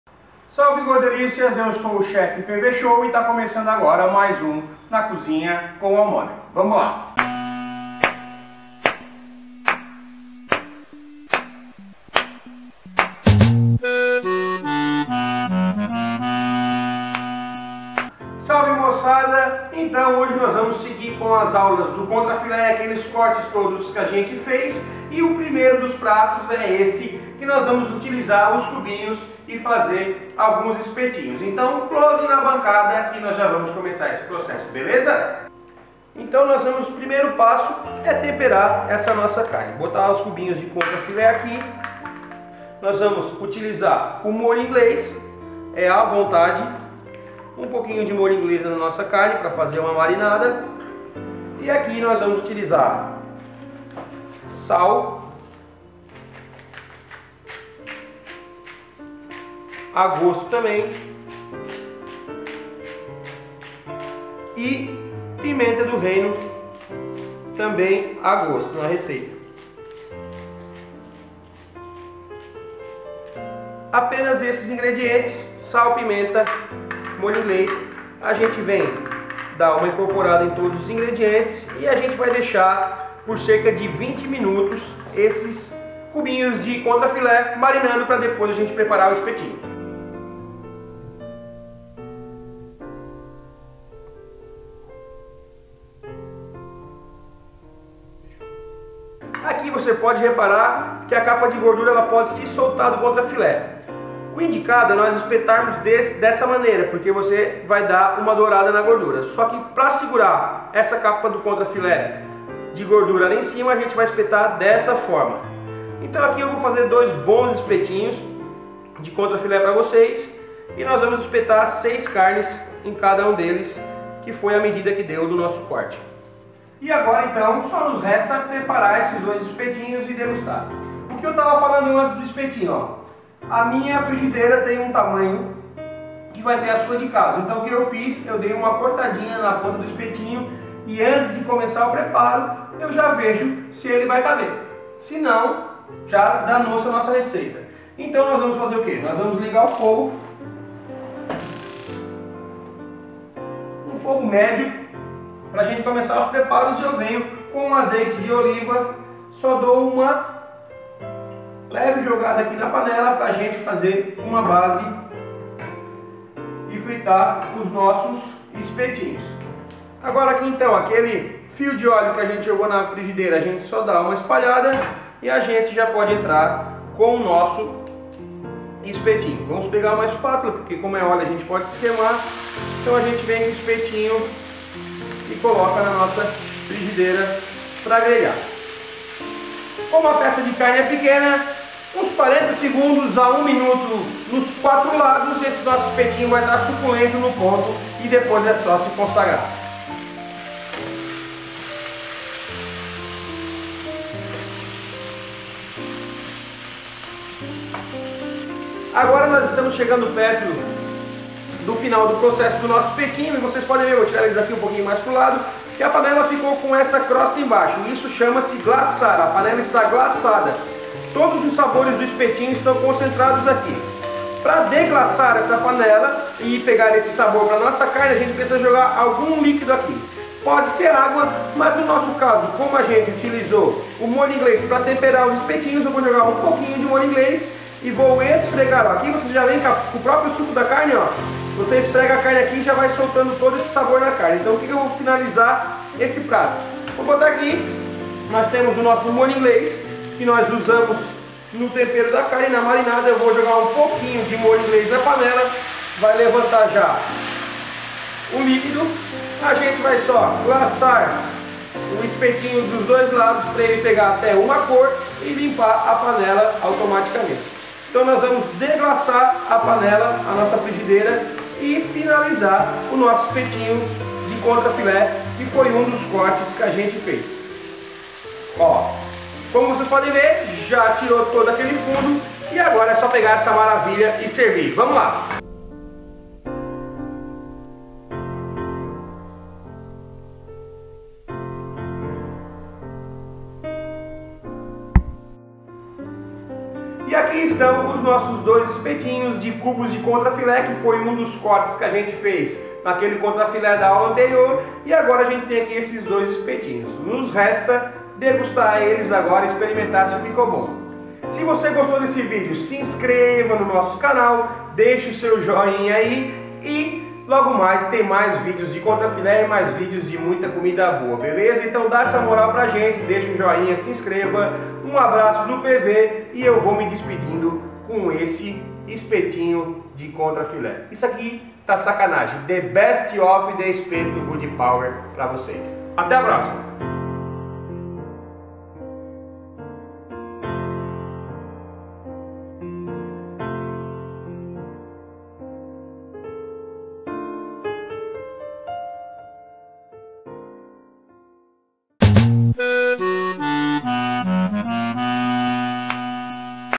COMO FAZER ESPETINHOS | tempero para espetinho
Aprenda a fazer espetinho de carne com o Chef PV Show. Ele usa o delicioso contra filé, que possibilita fazer um espetinho macio e muito suculento, preparado na frigideira, apenas com sal, ressaltando o sabor da carne.

Que aprender também a como temperar espetinhos, deixe nos comentários sua dúvida. No canal também temos uma receita de espetinho de frango feito na churrasqueira. É sensacional.

Ingredientes:

cubinhos de contra filé
molho inglês
sal
pimenta do reino
espetinho de madeira

E-MAIL PARA DÚVIDAS
NÃO CLIE AQUI!